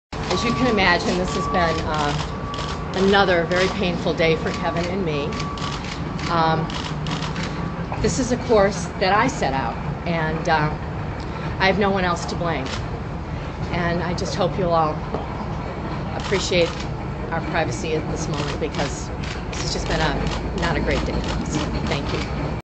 McCarty at court 01.16.09
Former Palm Beach County Commissioner Mary McCarty was released on $200,000 bond this morning after pleading not guilty to a federal corruption charge - the beginning of a legal process that she has said will eventually lead to a guilty plea and pris